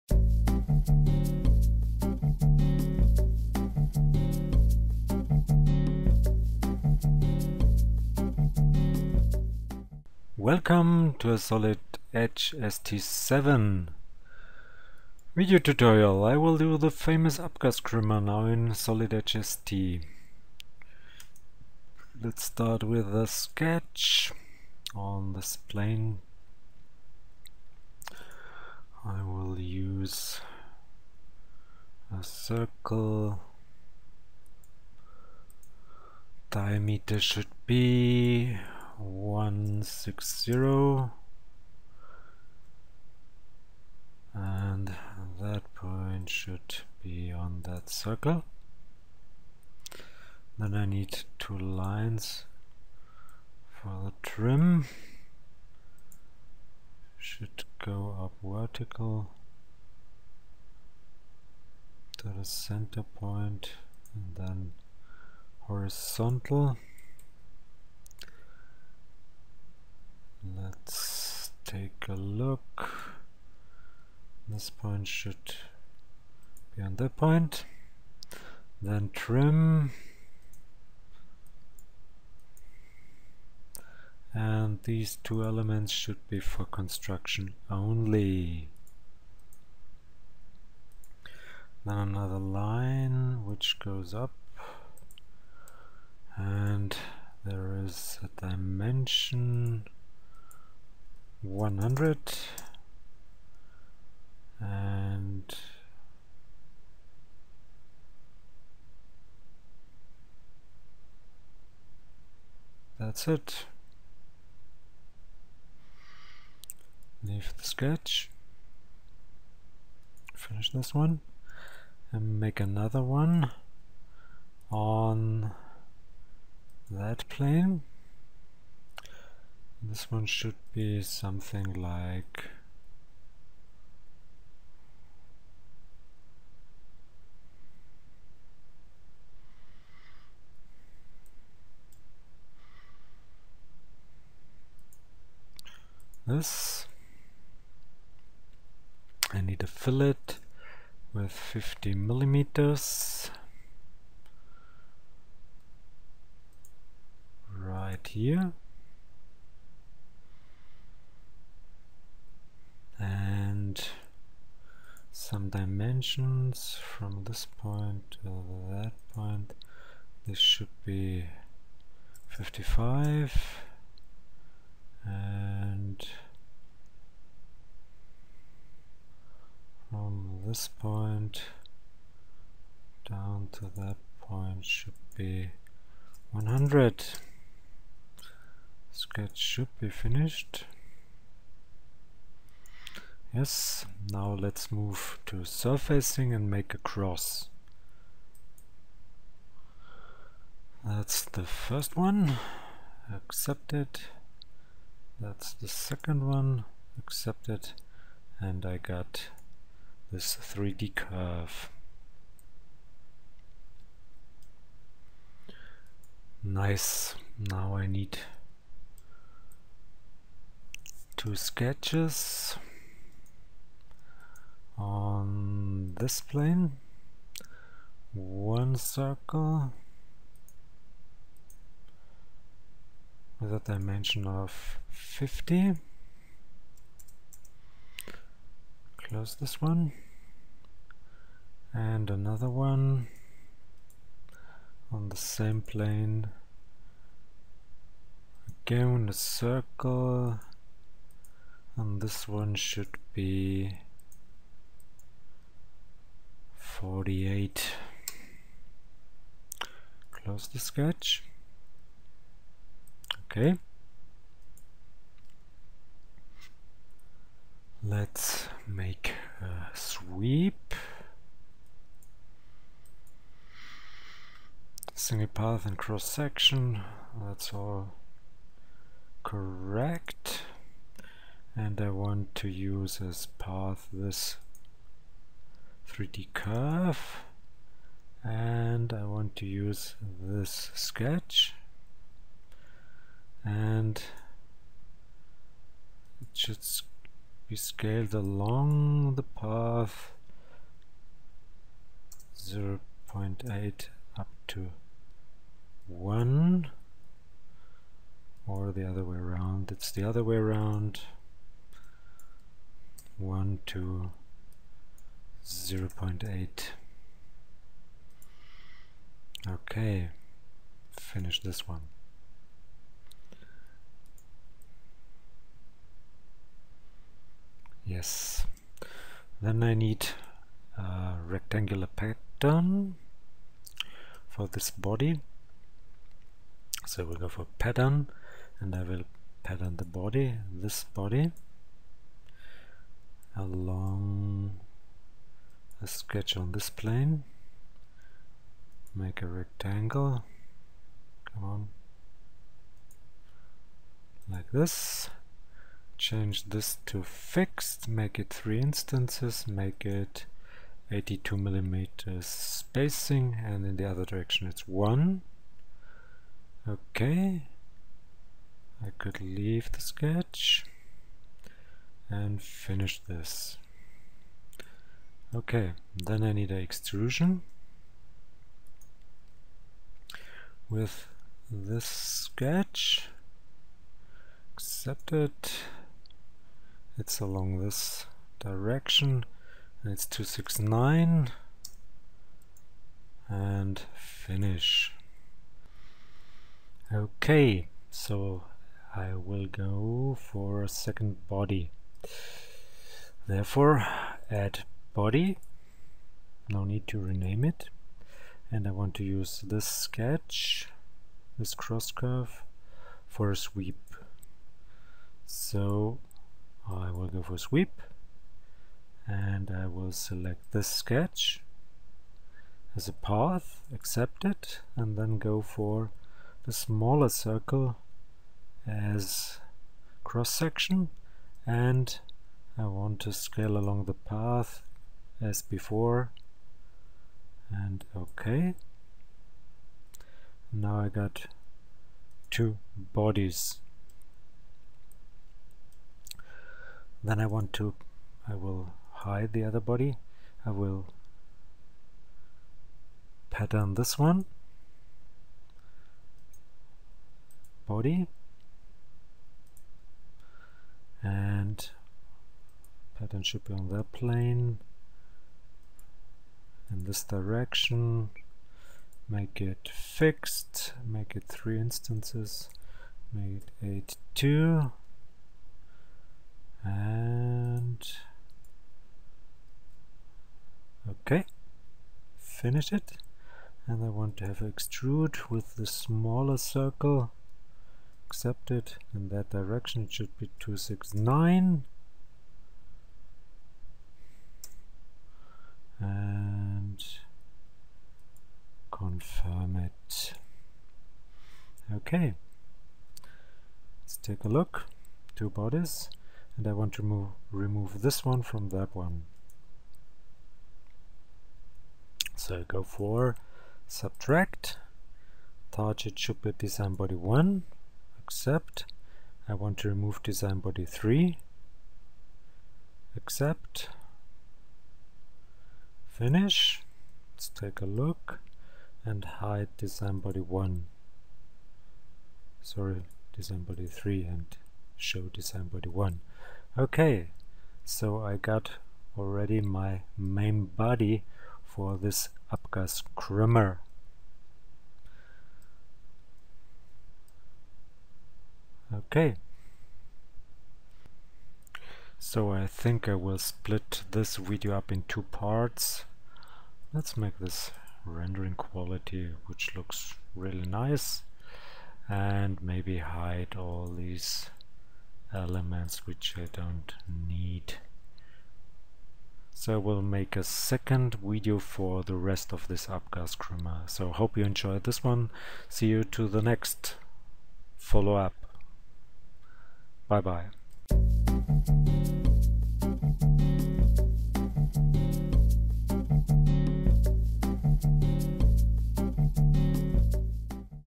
12.2 Abgaskruemmer part 1 - SolidEdge ST 7 Training - Loft - Multibody
i had to use a loft, a rectangular pattern and two bodies, otherwise I wouldn´t work. With sweep and shell - too complex for solidedge LOL. An exhaust manifold,

I split this tutorial in two parts - so take a look at the follow up for the rest of the exercise.